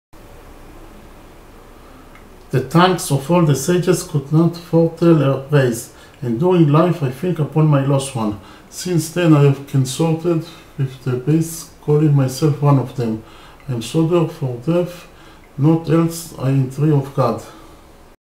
SHOTA RUSTAVELI
Shalom Barel reads one stanza from the great and gorgeous poem:
The Knight in the Panther's Skin.
About : P’HRIDON TELLS TARIEL TIDINGS OF NESTAN-DAREDJAN.